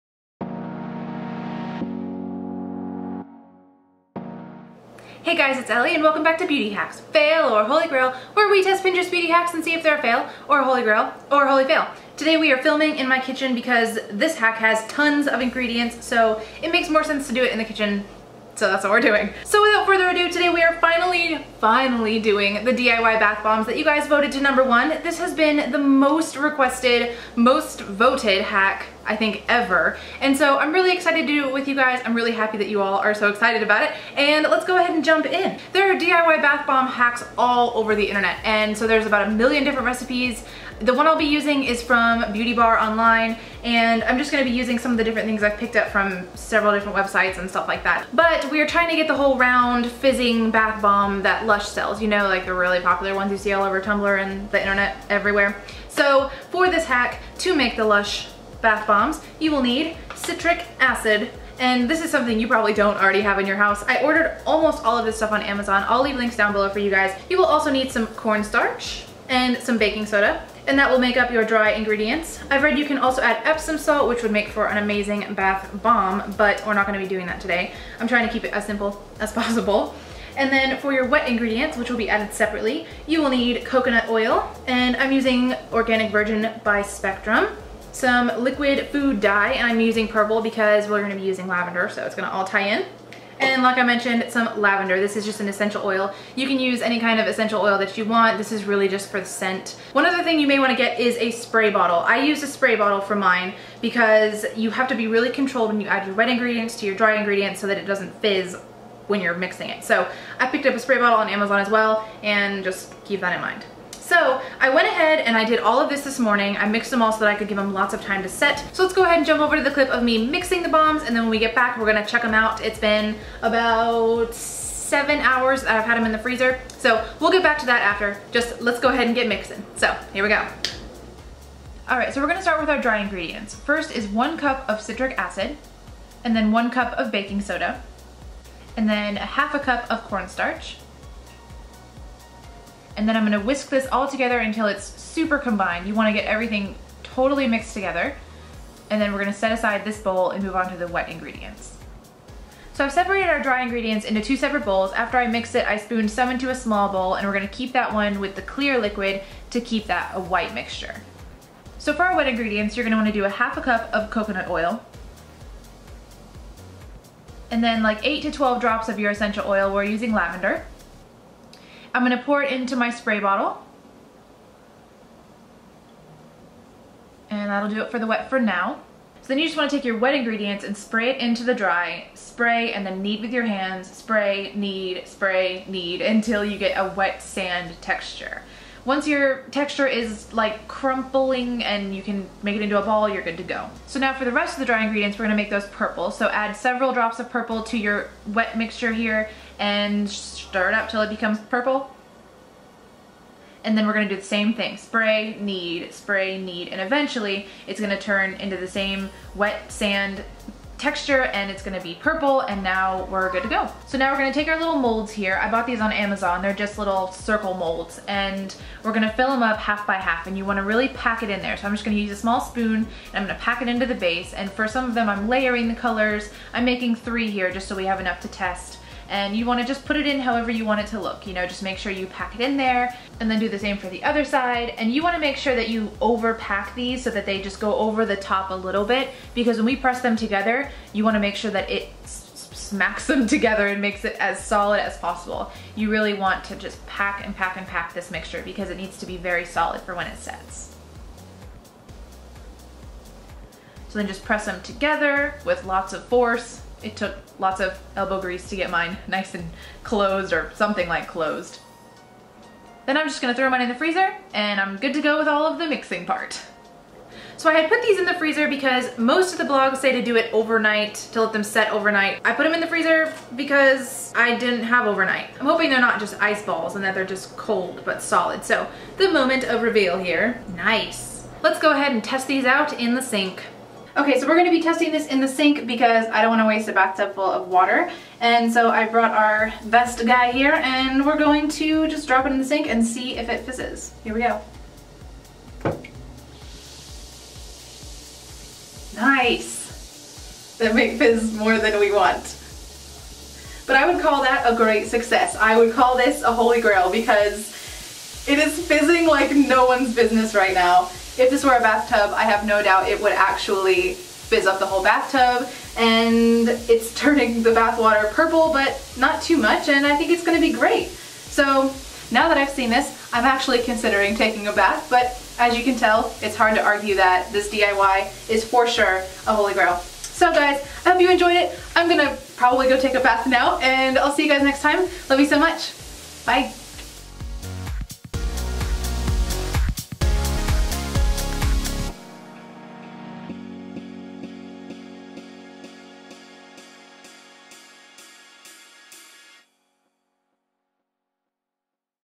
Beauty Hacks: Fail or Holy Grail? ♥ DIY Bath Bombs | Ellko
The DIY "Lush" Bath Bomb craze has been wild lately and you guys voted this request to the very top! Finally it's time to see if this pin is a Fail or a Holy Grail!

xo Ellie

*Ingredients:
Round Molds
Citric Acid
Baking Soda
Corn Starch
Coconut Oil
Essential Oil
Food Coloring